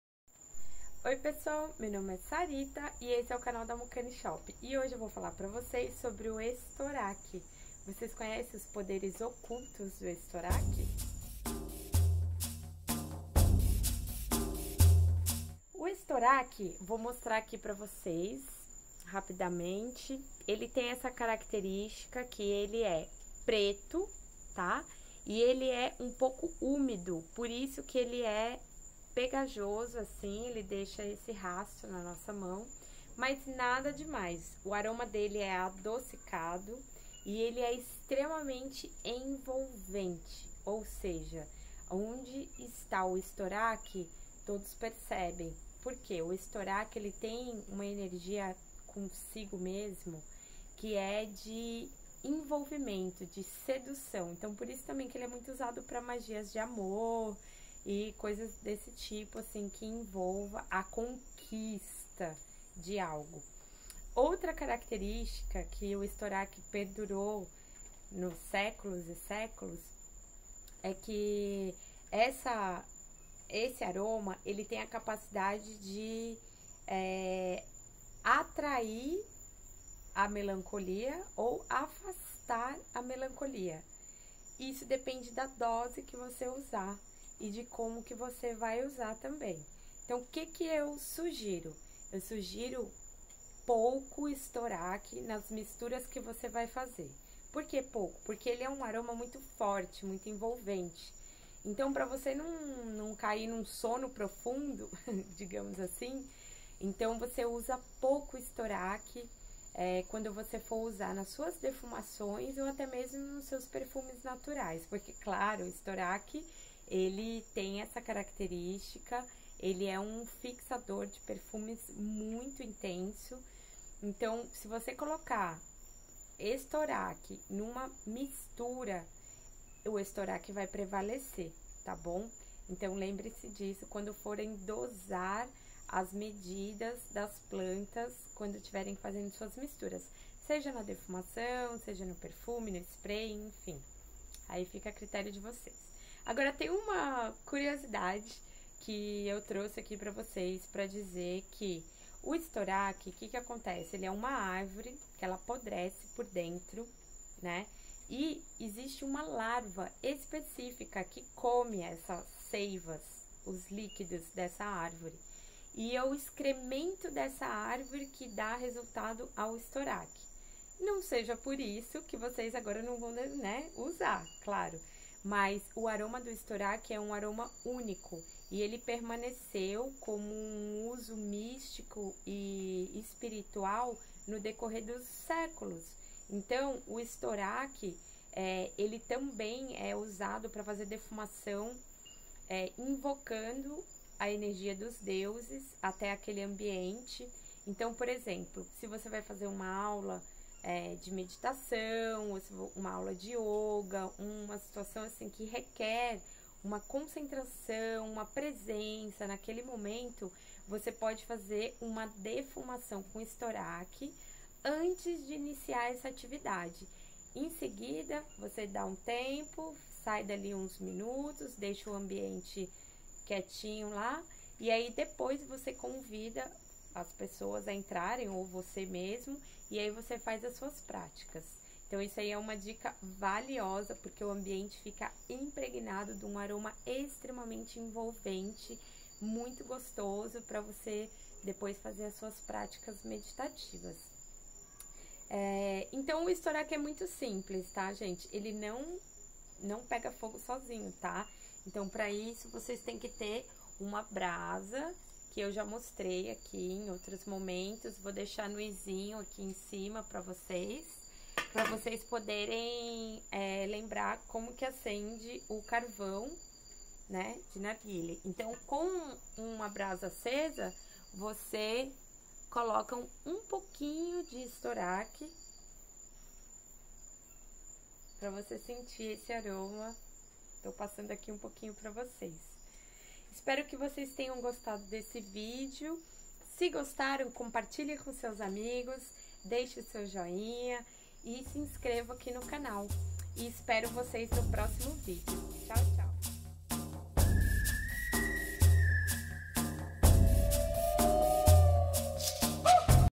Estoraque e seus poderes ocultos
USOS MÁGICOS: Esta resina é utilizada para momentos de oração aos Anjos, relaxamento, fortalecimento, amor, sensualidade, ajuda com o sono.

INSTRUÇÕES DE USO: Acenda um carvão vegetal e jogue algumas resinas sobre a brasa. A resina irá derreter liberando sua fragrância no ar por vários minutos. Após este tempo, use o pegador para remover os resíduos da brasa e caso desejar, adicione mais resina.

NOME CIENTÍFICO: Liquidambar orientalis

ORIGEM: Turquia

OBSERVAÇÕES: Este produto é 100% natural, portanto poderá sofrer alterações em relação às fotos apresentadas no site, podendo variar cor, tamanho exato, entre outras caracteristicas.